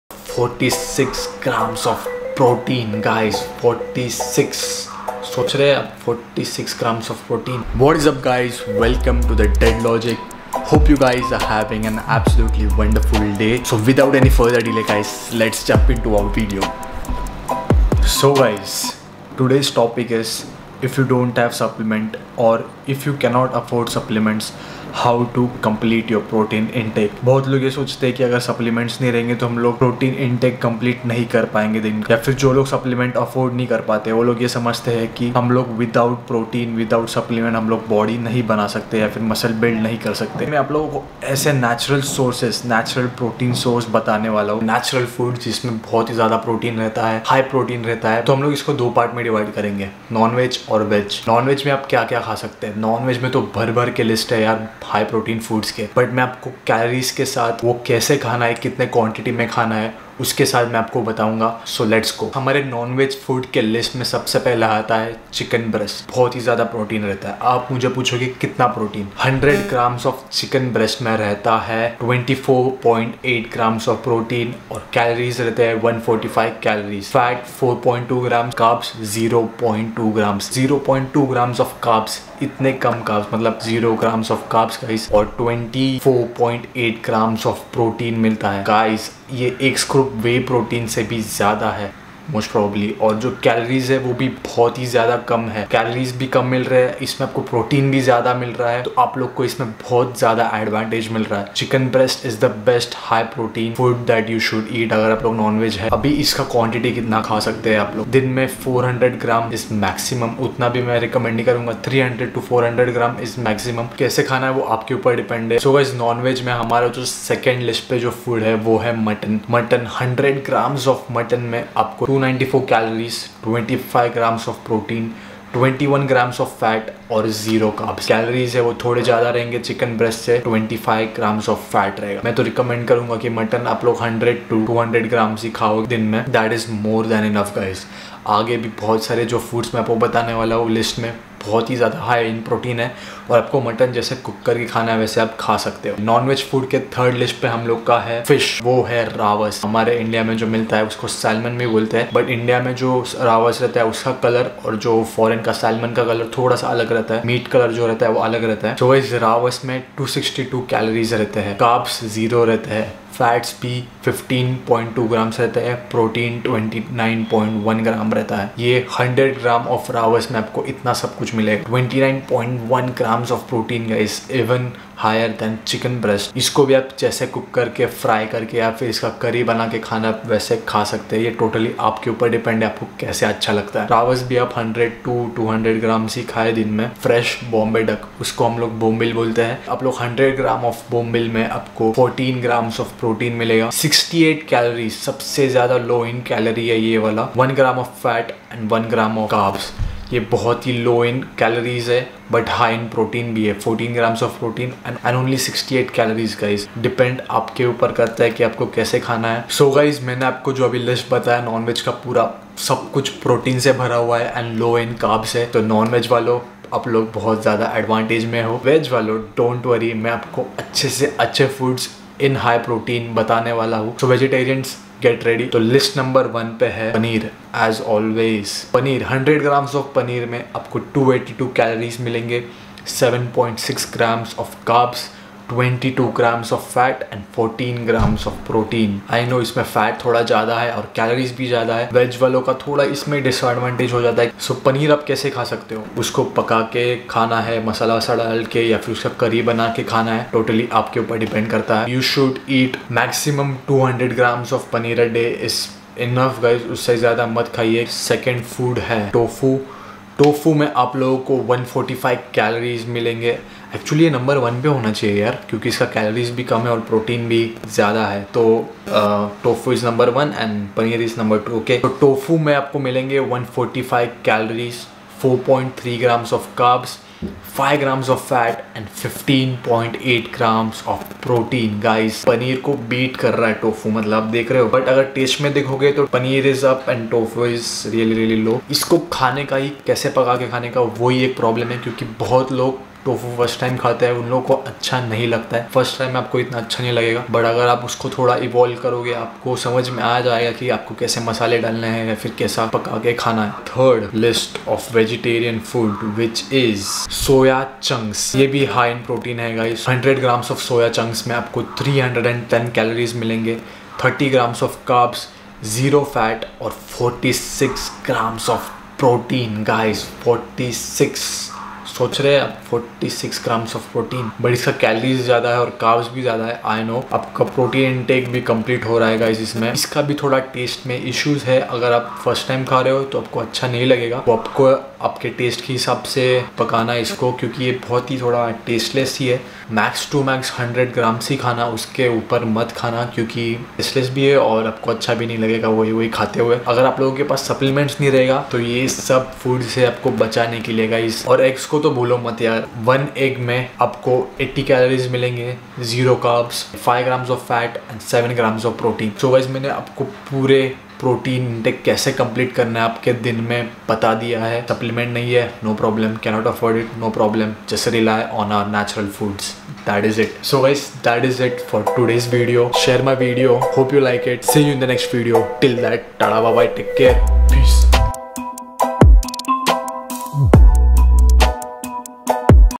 CAN'T AFFORD SUPPLEMENTS | NATURAL PROTEIN SOURCE UNDER Rs. 500 | Fitness Series E 11
Hey guys, back again with a new video. In this video, I have talked about natural protein source, the once who can't afford protein can complete their protein intake with natural protein source. Hope this video was helpful.